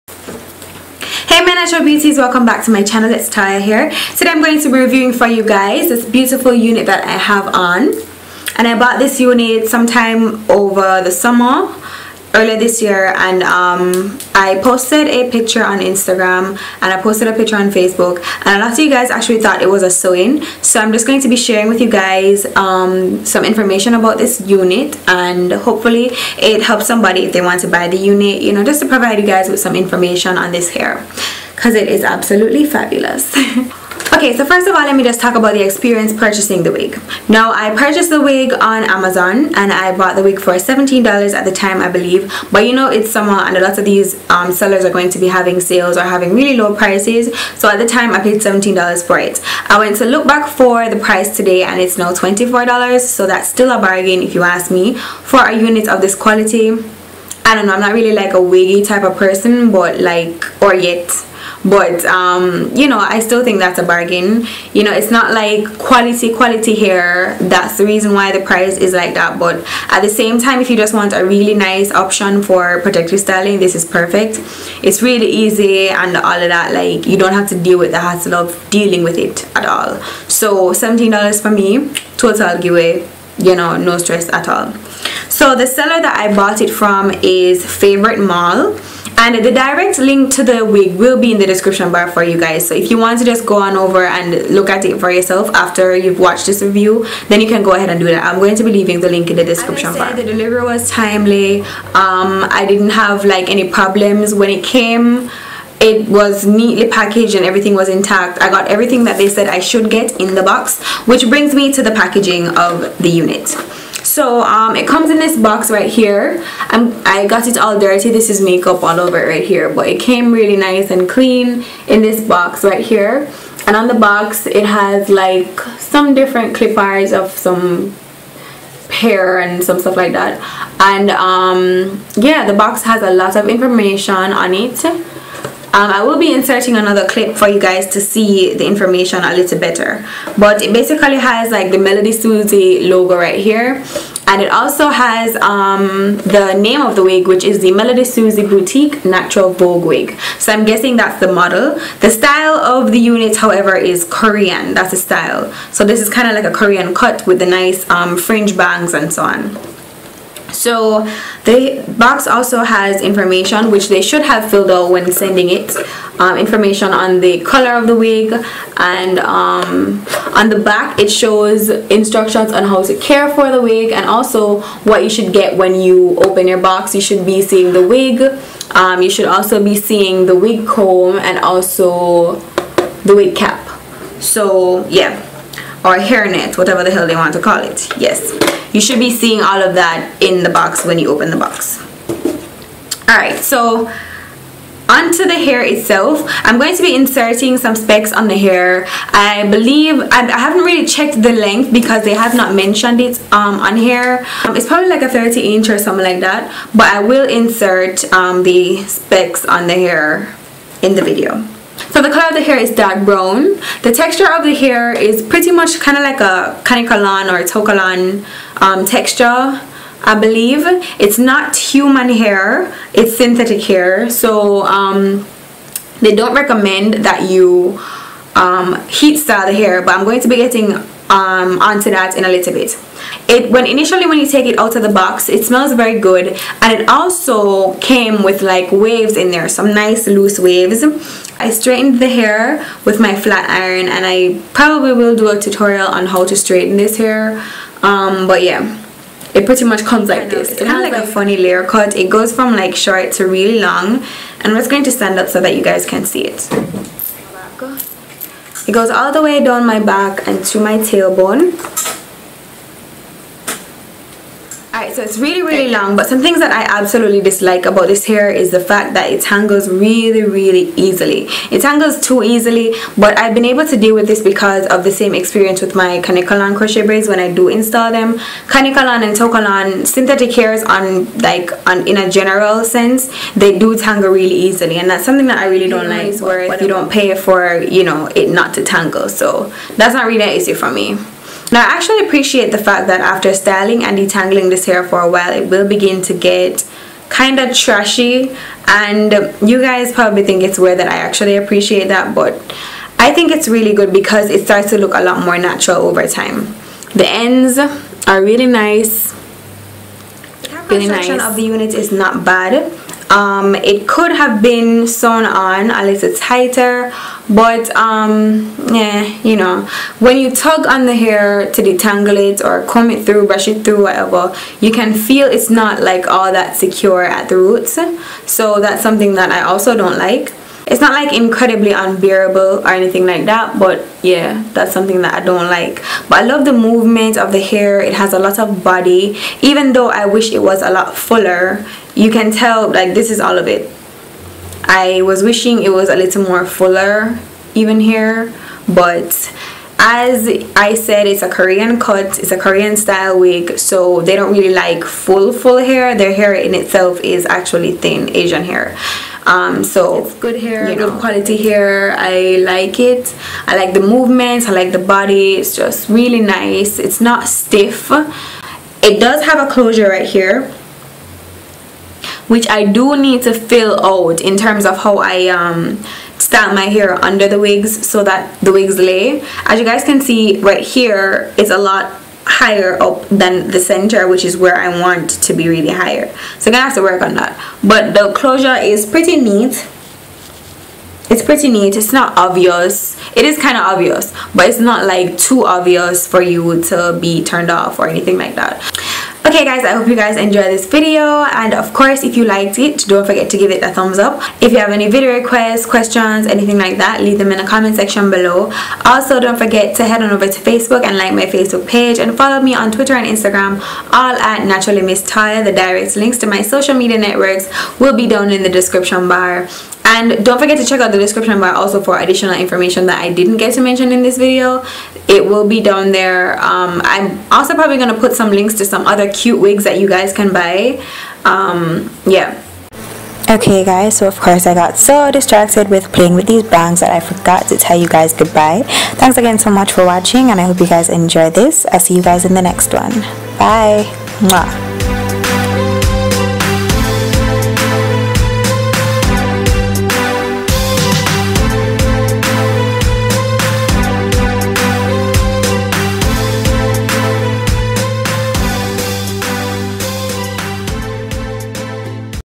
Sexy Korean Hair! Melody Susie Wig Review / Show &Tell
WIG SPECS:
Length: 29"
Hair type: Silky straight/ wavy
Cut Type: Korean (Toyokalon Synthetic hair)
Cut: Layered w/ Fringe Bangs
Colour: Dark Brown
Wig construction: Full wig w/ closure

LINK TO THE WIG

OTHER CUTE WIGS